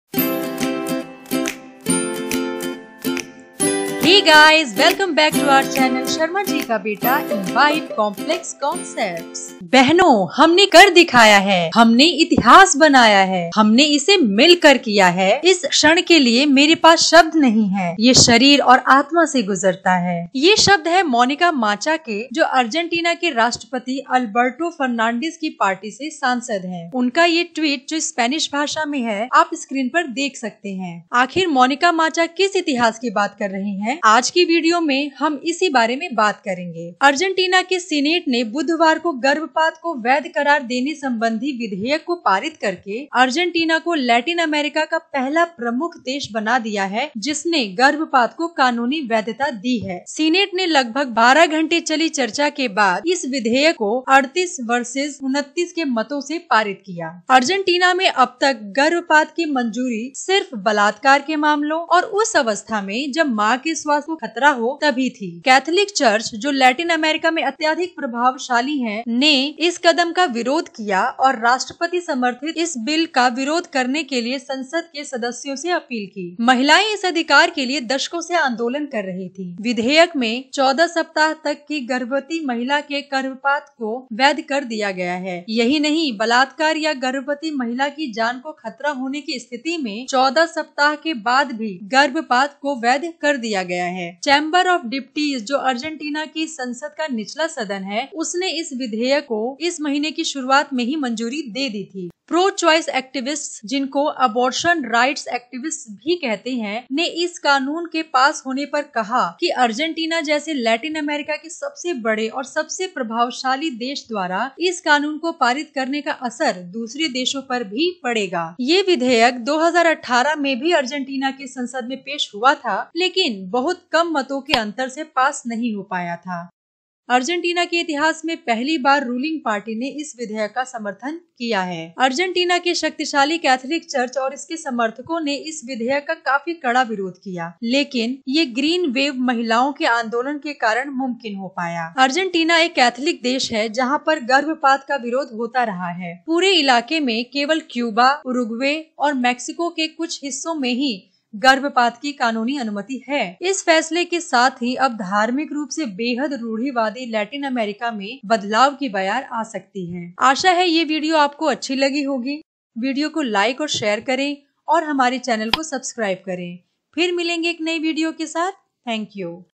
Argentina became the largest country in Latin America to legalise abortion | Abortion Rights
Argentina's Congress has legalised abortions up to the 14th week of pregnancy, a ground-breaking move for a region that has some of the world's most restrictive termination laws.